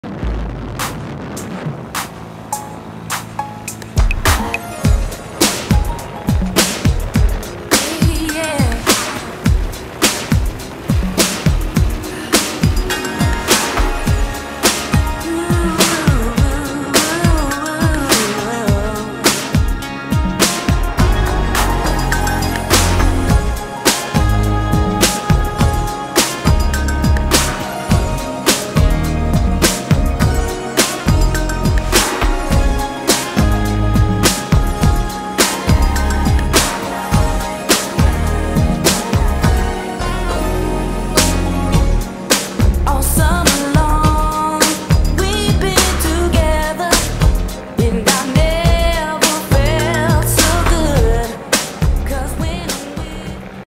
Short paramotor vid from 15/07/14 near Selby with Beaker and Slater one of the smoothest nights ever
via YouTube Capture flown with Beaker and Slater near Burn, Selby all on Flymecc paramotors, me and Beaker using Jojo wings, perfect night all in all great flight